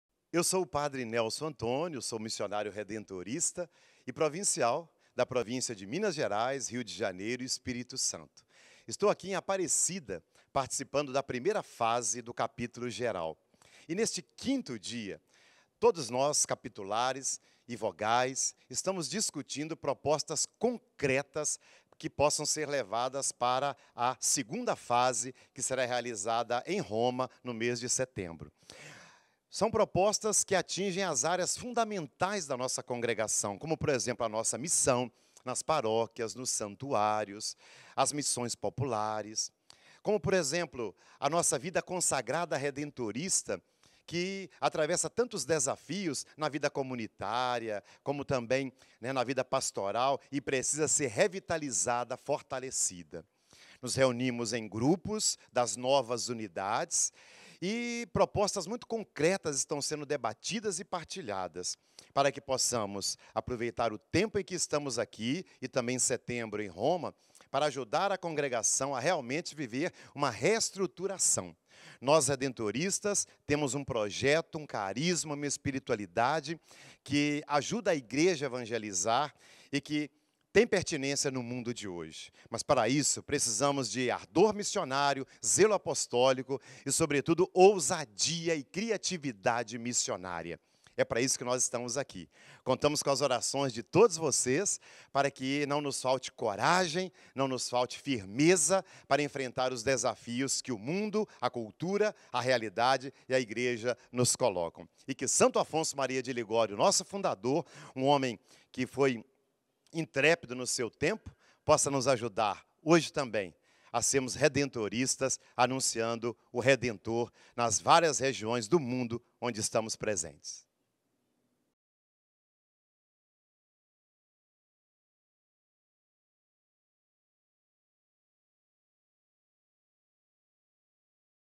Padre Nelson Antônio Linhares, C.Ss.R. relata sobre o quinto dia do 26º Capítulo Geral
Provincial do Rio de Janeiro, Pe. Nelson Antônio Linhares, destaca alguns temas de discussão do quinto dia do 26º Capítulo Geral, que acontece em Aparecida (SP).